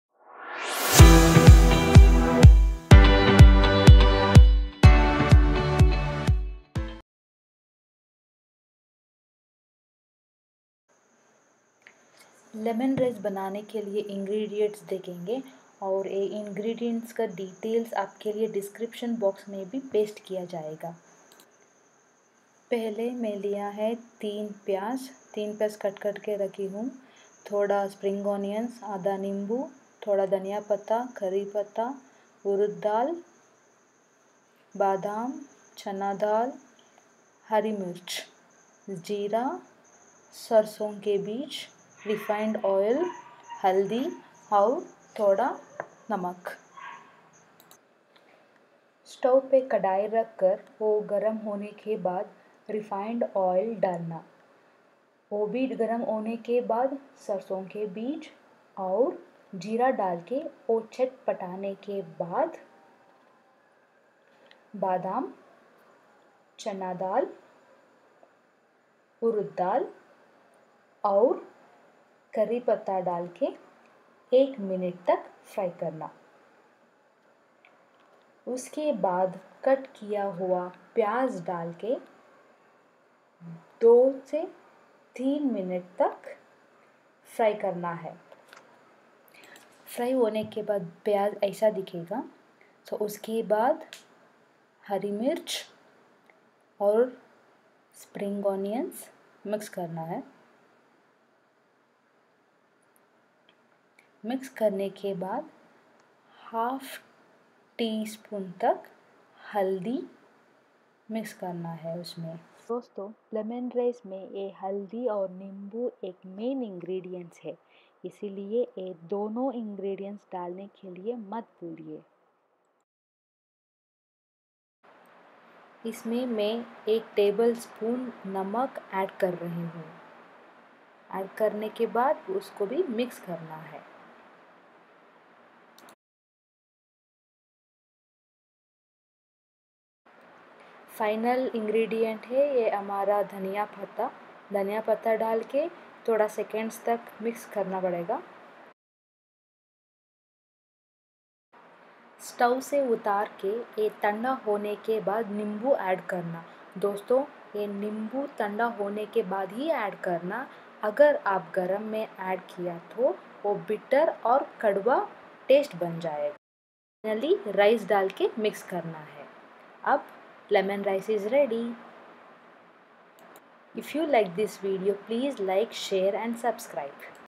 Lemon Rice For North Indians In Simple Steps | South Indian Special | Hindi With English Subtitles.
Hey Guys!!!
This is the south indian authentic food made especially for North indians to ease their effort for making breakfast in the morning peak time.
This can be prepared with the left over rice also and can be done in 10-15mins.

Ingredients required:

Onions  -- 3
Lemon  -- Half
Coriander Leaves  -- 5 - 6 strands
Curry Leaves  --  2 - 3 strands
Channa dal  --  1 table spoon
Urad dal  --  1 table spoon
Badam  -- 2 table spoons
Green chillies  --  4
Spring onions  --  Half a cup (Optioinal)
Mustard seeds  --  1 tea spoon
Jeera  -- 1 tea spoon
Refined oil  -- 4 -5 spoons